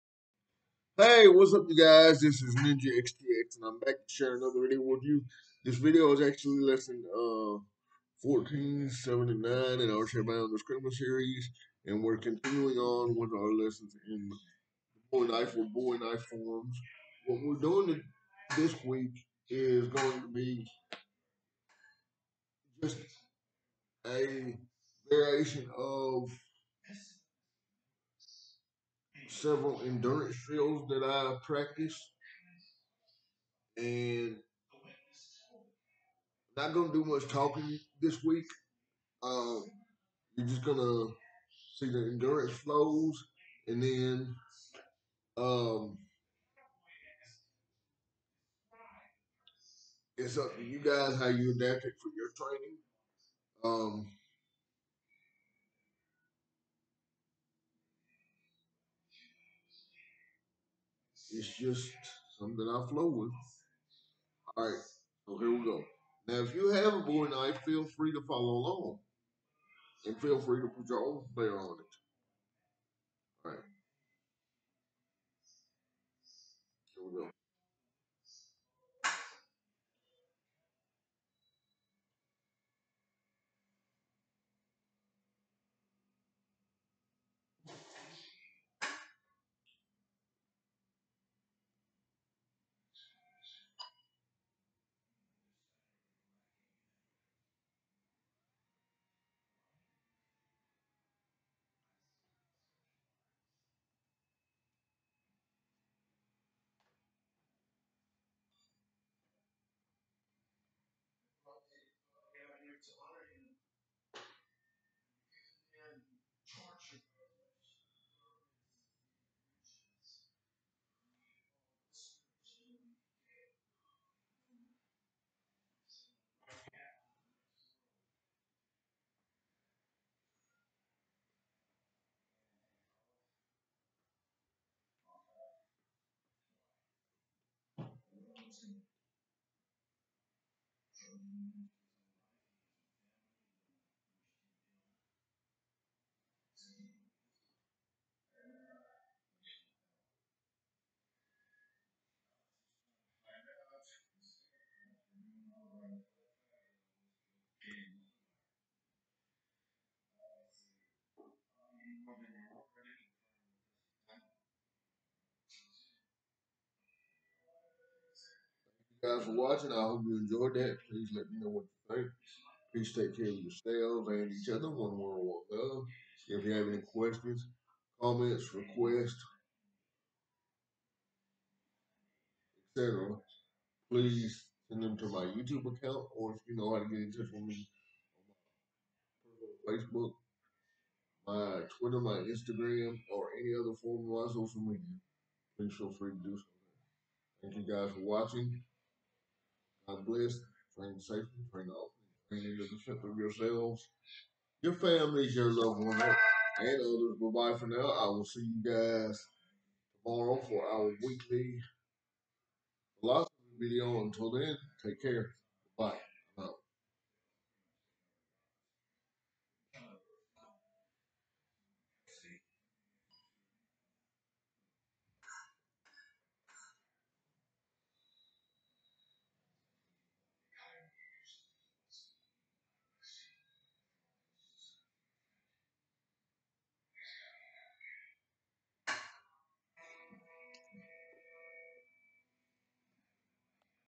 Chairbound Escrima Lesson 1479 4-11-2022
Chairbound Escrima Lesson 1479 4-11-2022  This lesson focuses of Part 45 Of Bowie Knife Forms. Please Enjoy, Give Feedback And Remember To Practice Practice Practice